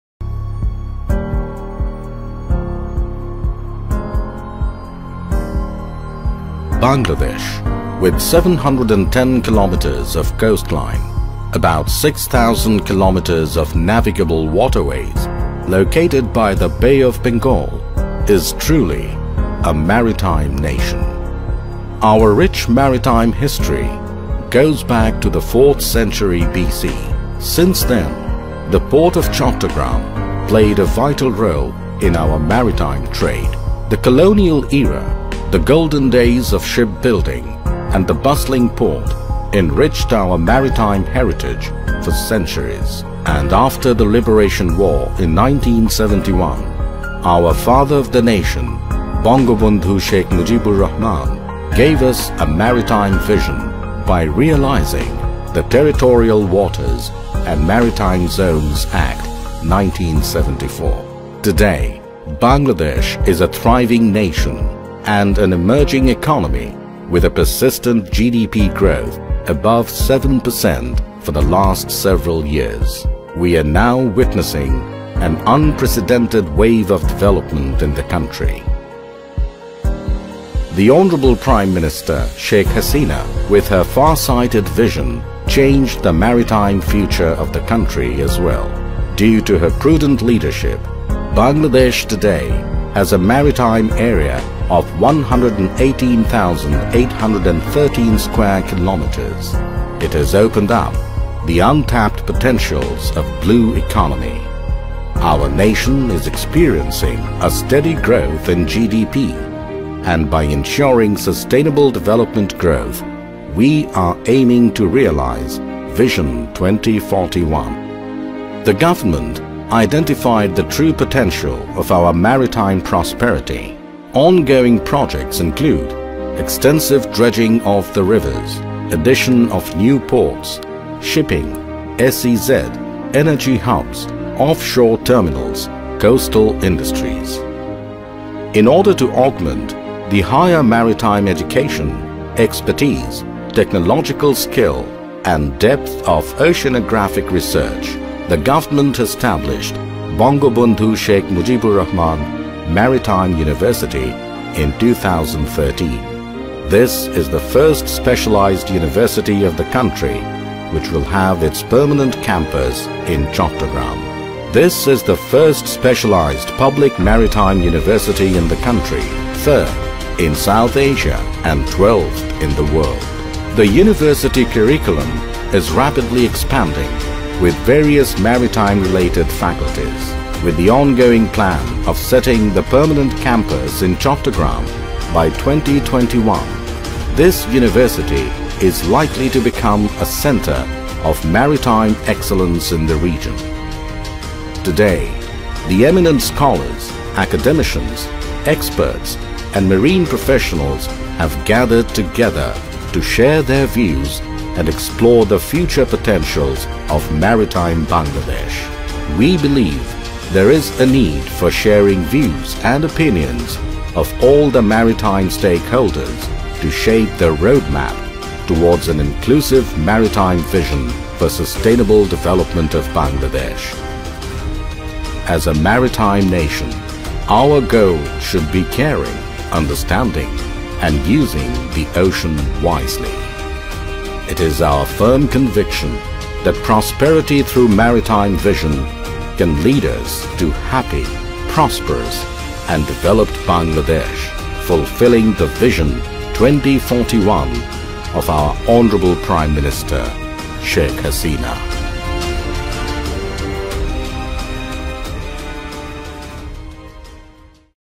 About BSMRMU Maritime University || About Bangabandhu Sheikh Mujibur Rahman Maritime University
Hi viewers here I have shown About Bangabandhu Sheikh Mujibur Rahman Maritime University/ BSMRMU.This channel is all about Marine related information, technology and vlogging.